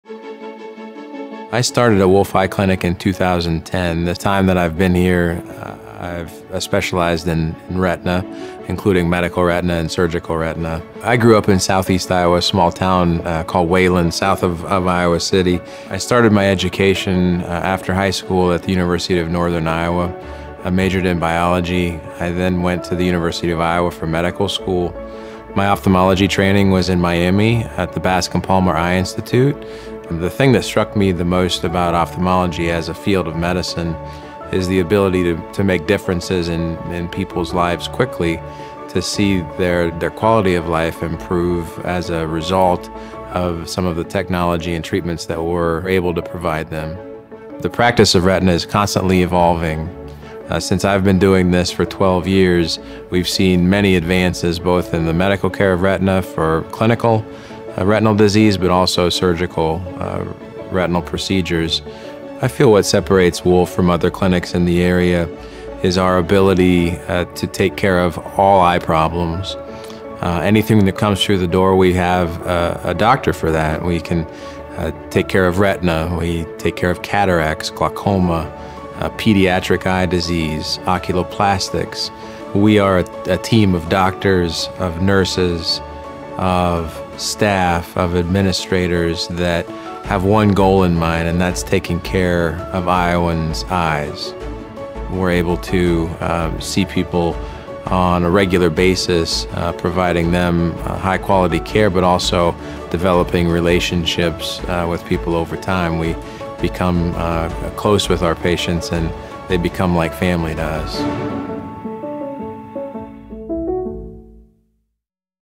Meet Dr. Kyle Alliman | Wolfe Eye Clinic Retina Specialist in Des Moines, Iowa
Meet Dr. Kyle J. Alliman, a fellowship-trained retina specialist focusing on diabetic eye disorders, macular degeneration and surgical retina repair in Iowa. Dr. Alliman has been seeing patients with Wolfe Eye Clinic since 2010, and he enjoys caring for his patients and giving those with previously untreatable eye diseases the ability to maintain their vision and quality of life.